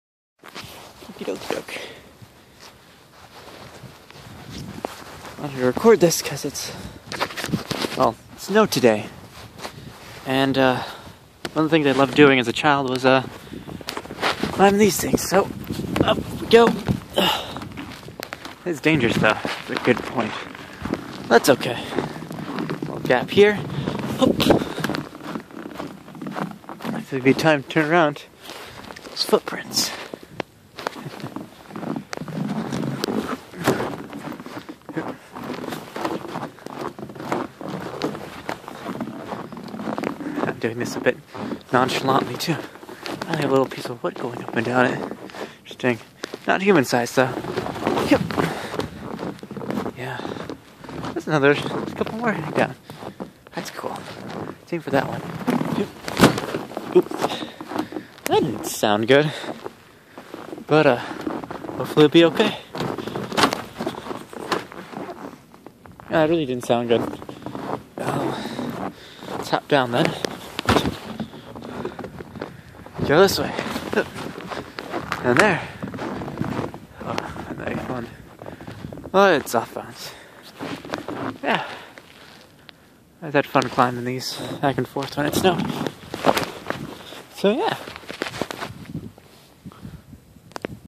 Let's Play "It Actually Snowed Yesterday!"
Realistic First-Person Platforming!

It was snowing yesterday, but I was working out (and then animating for way too long), so I got to take some videos today. ^^ (If you thought this one was okay, I could upload the other one. ^^; )